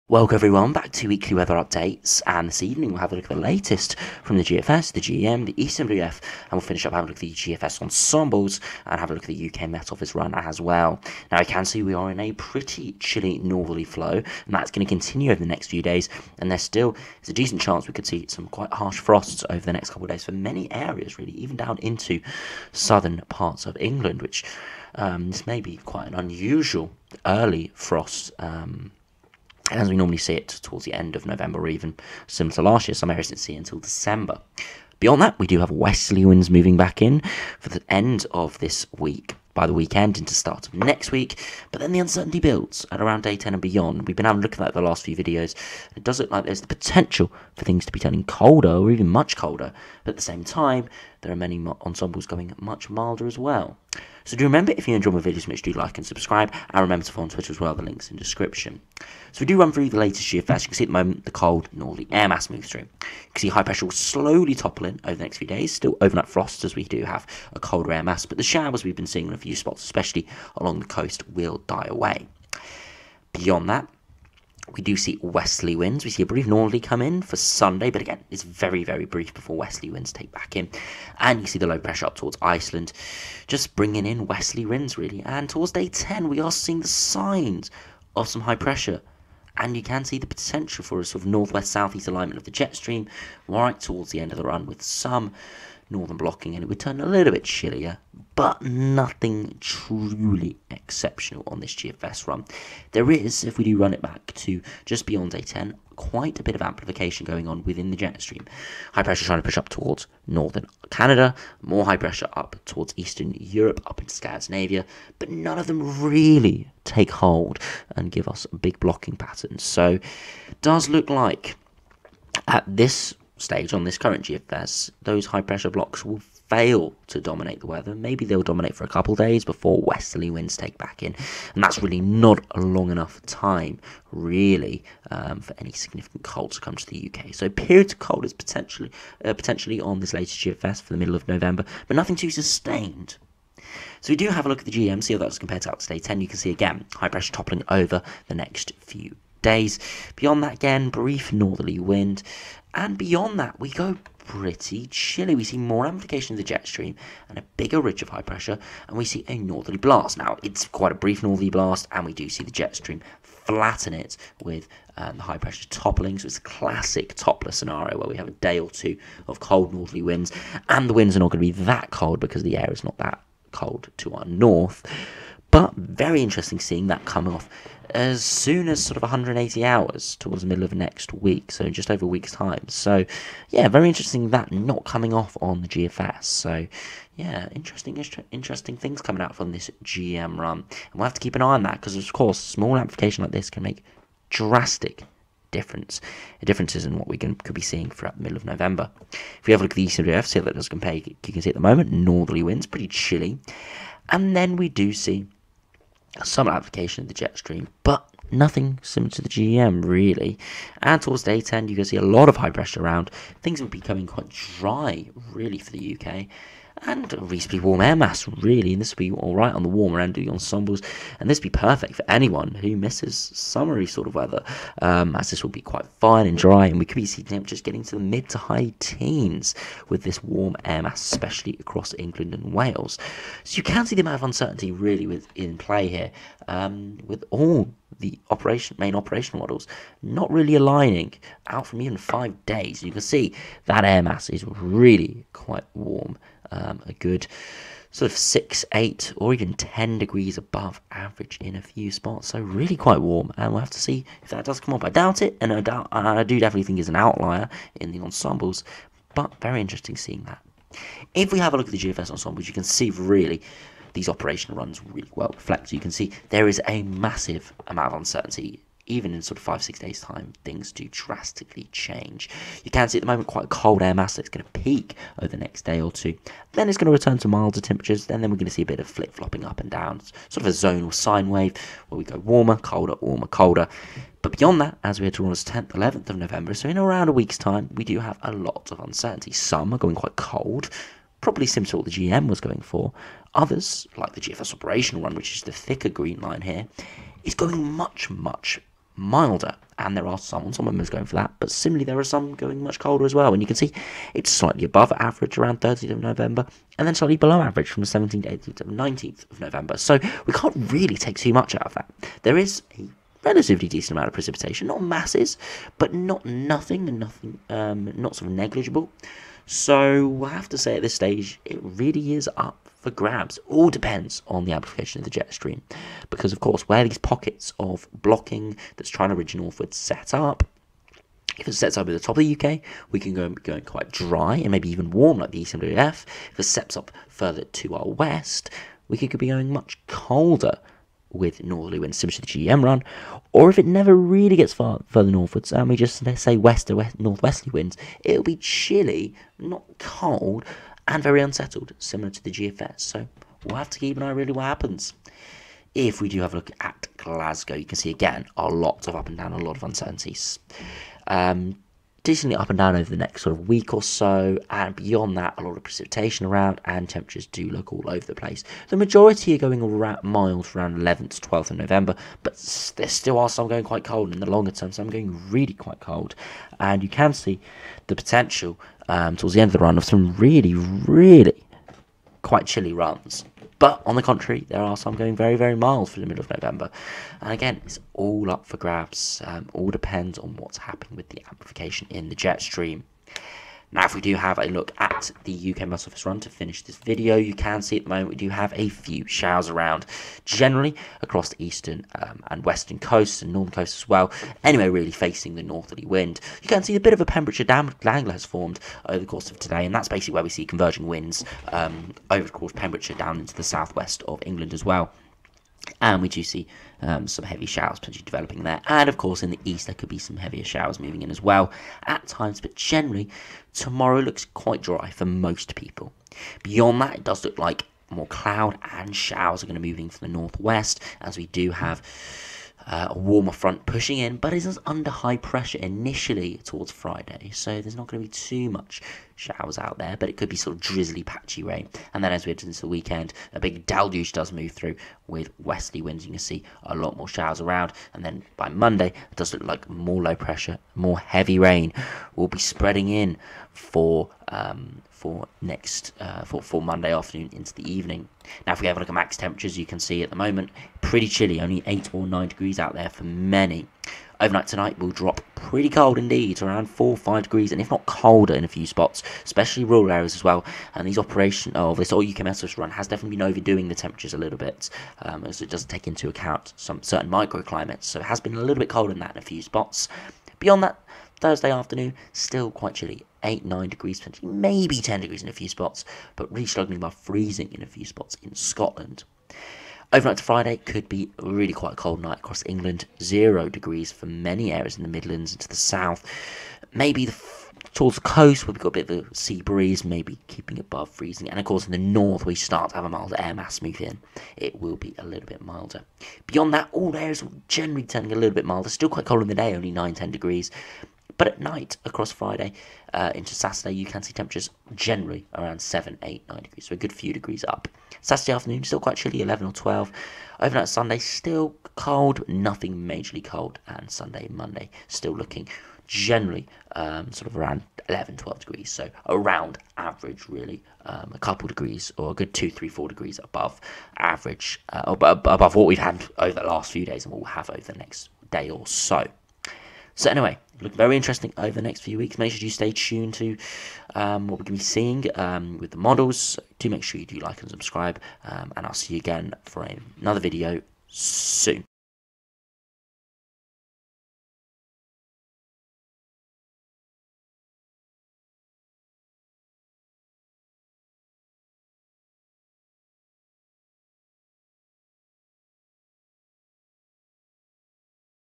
Big Model Disagreement for Next Week! 3rd November 2021
00:00 GFS Operational Run
02:25 GEM Operational Run
03:20 ECMWF Operational Run
04:22 GFS Ensembles
06:35 UKMO (High Resolution) Precipitation Charts